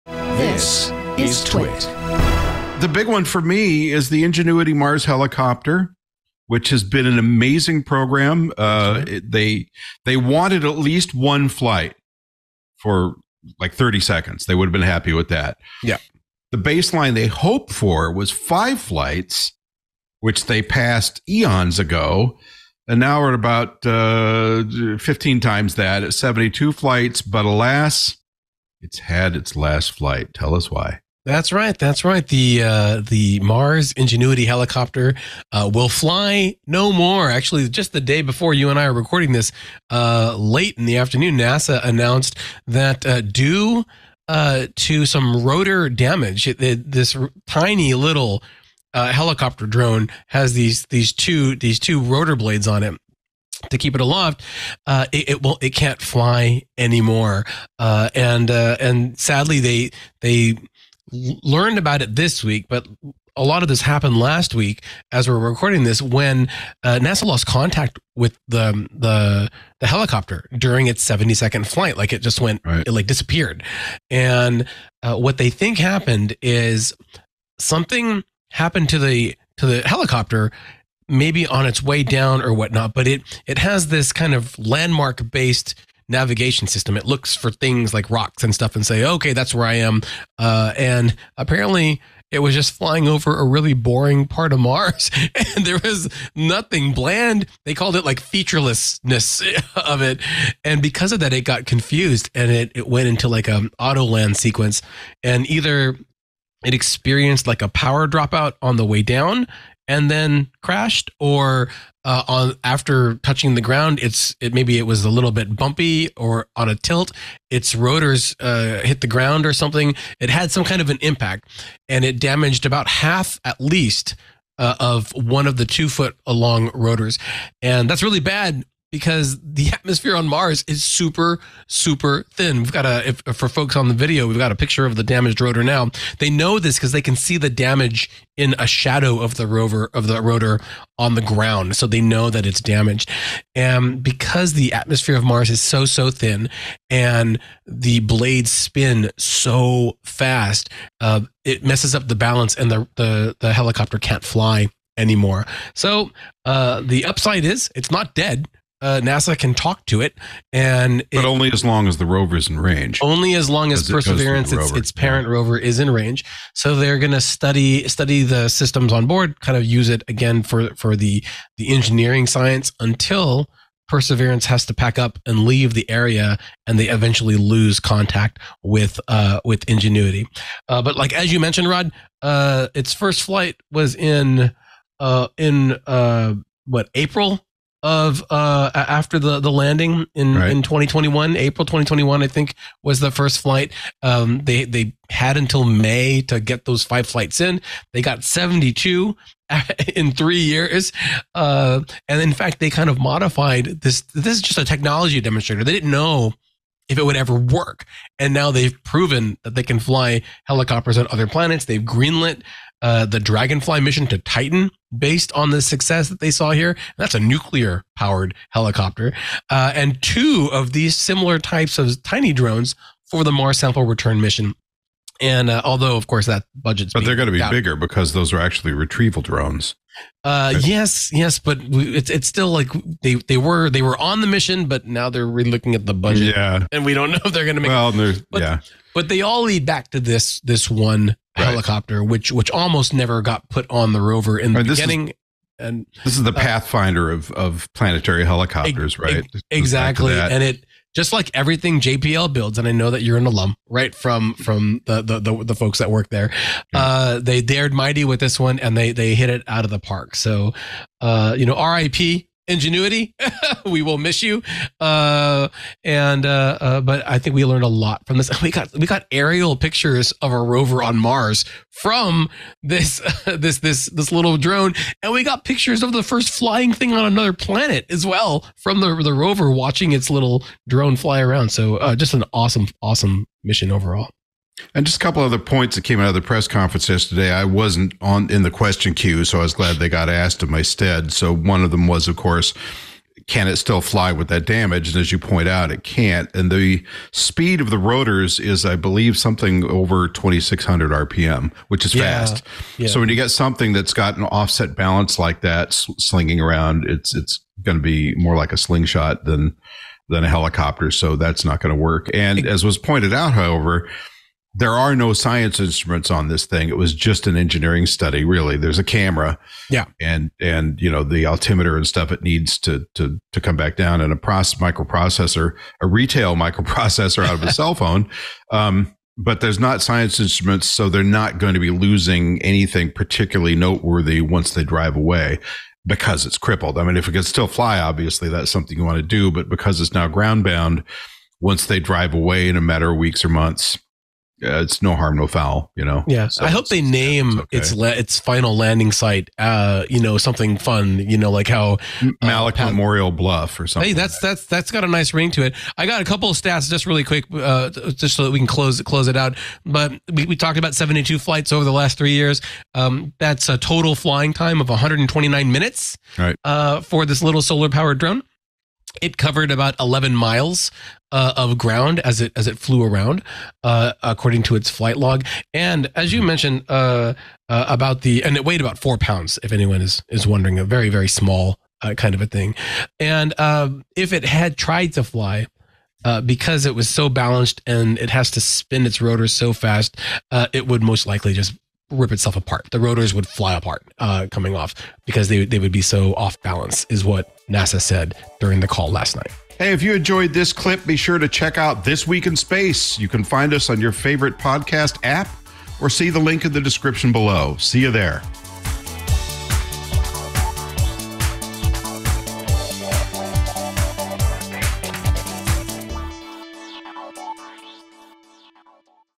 Mars Ingenuity's Final Flight: A Look Back at Its Achievements
Explore the incredible journey of the Mars Ingenuity Helicopter in its final flight. Hosts Rod Pyle and Tariq Malik discuss the achievements, challenges, and the lasting impact of this groundbreaking mission on future space exploration. Want more space insights?